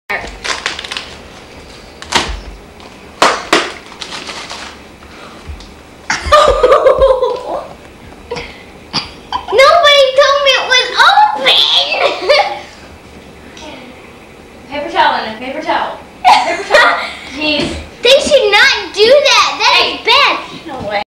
Alana Cooks Corn Muffins in the 90's - Part 1
My sister used to make her own "cooking shows" back in 1995. This is part 1 of this particular disaster.